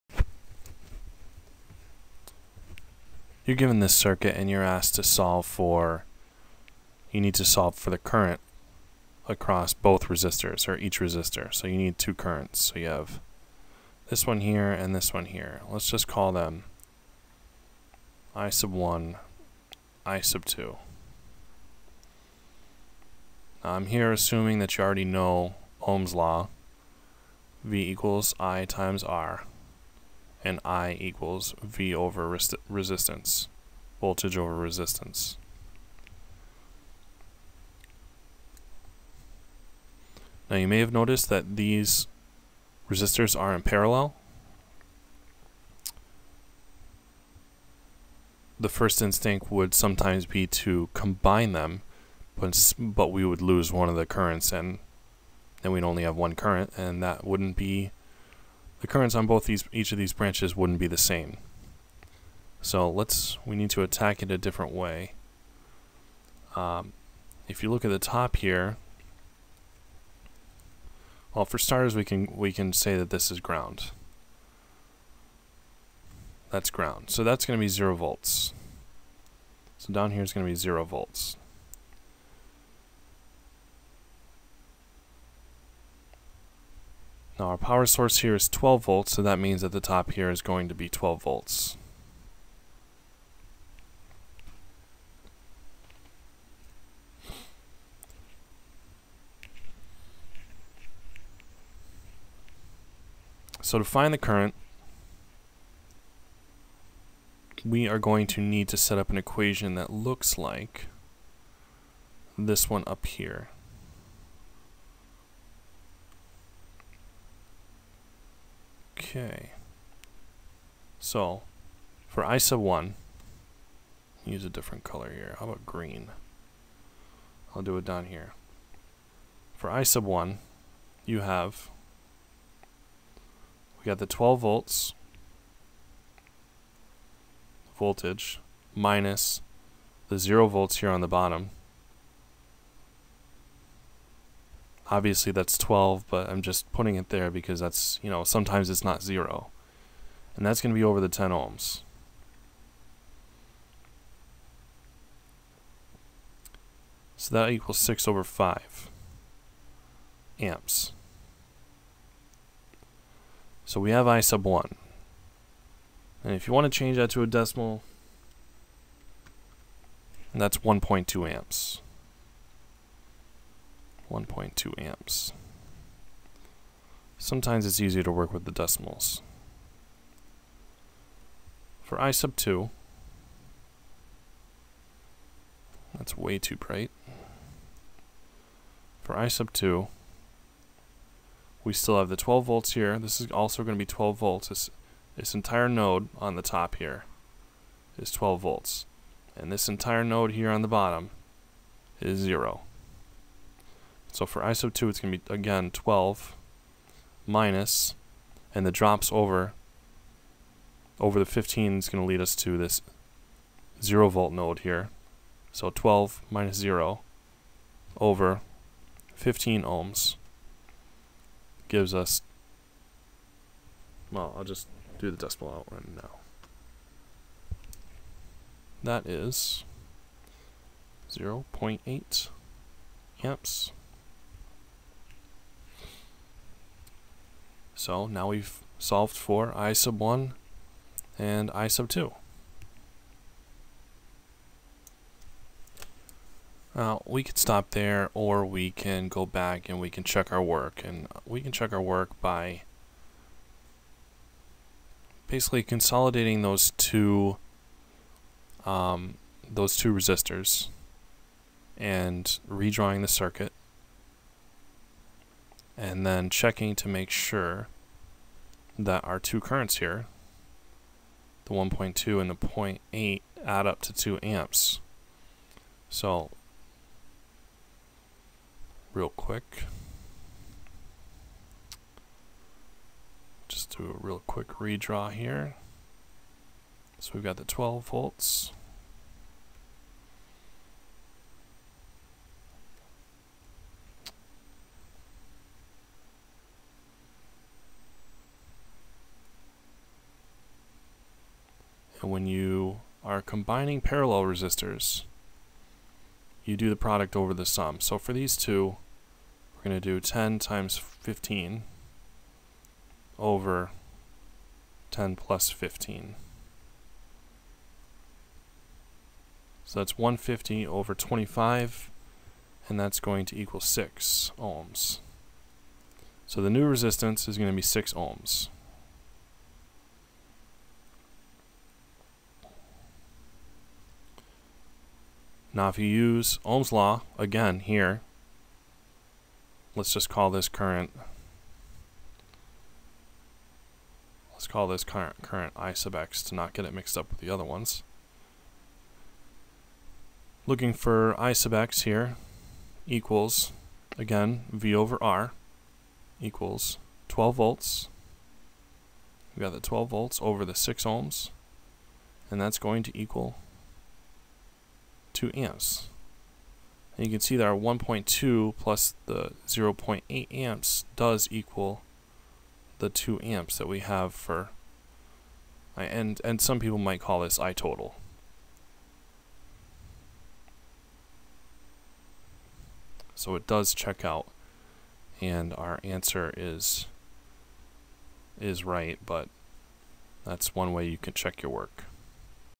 How to Find Current in a Parallel Circuit
Analysis of a circuit containing two resistors in parallel. Solving for the current across each resistor, as well as the total current.